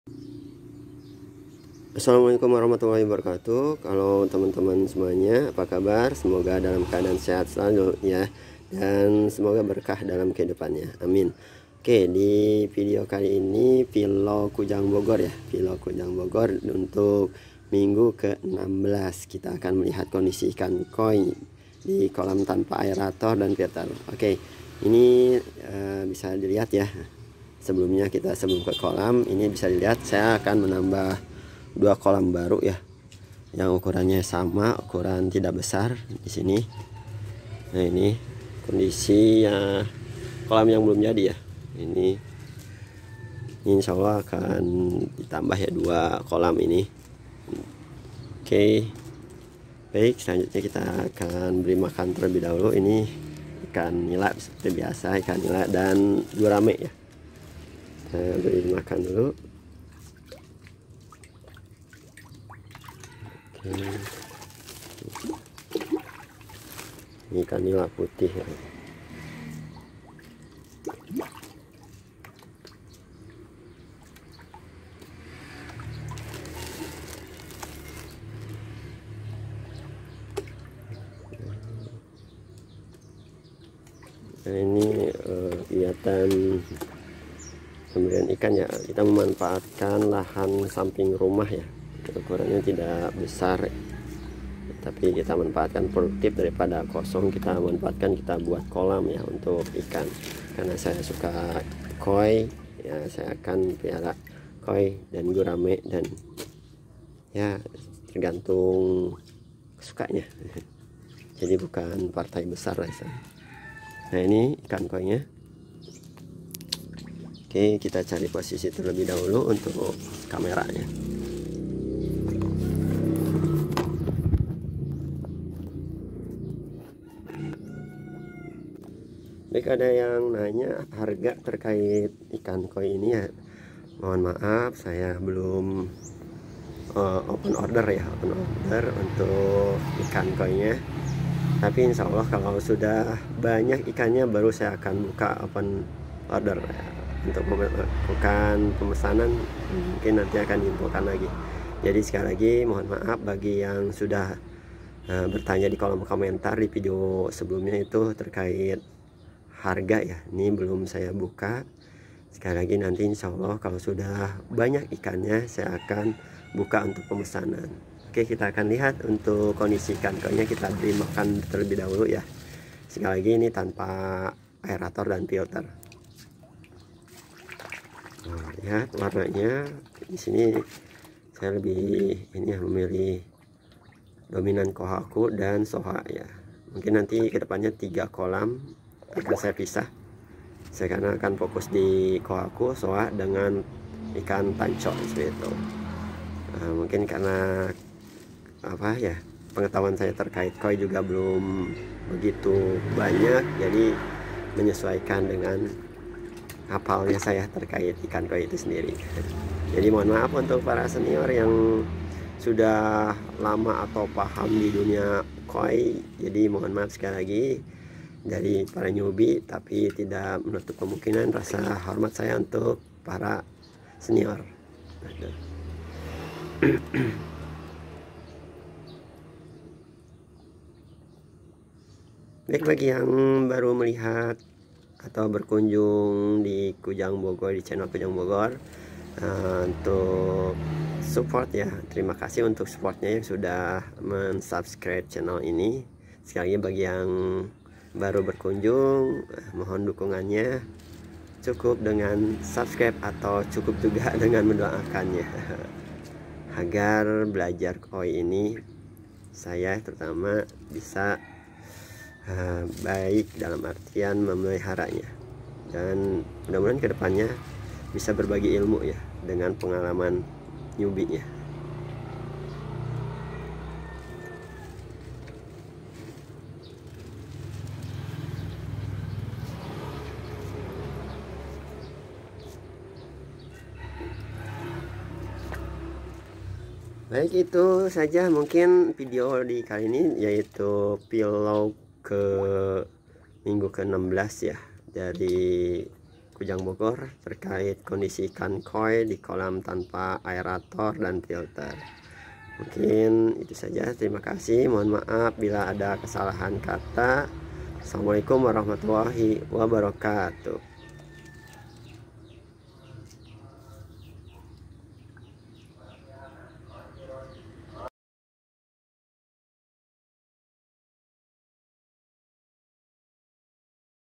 Kondisi ikan Koi setelah Enam Belas Minggu di Kolam tanpa filter dan aerator | DailyVlog
Assalamu'alaikum. Halo semuanya apa kabar, semoga dalam keadaan sehat dan berkah selalu ya. DailyVilog kali ini kita akan melihat Kondisi ikan Koi setelah Enam Belas Minggu di Kolam tanpa filter dan aerator. Ok, silahkan melihat. 😄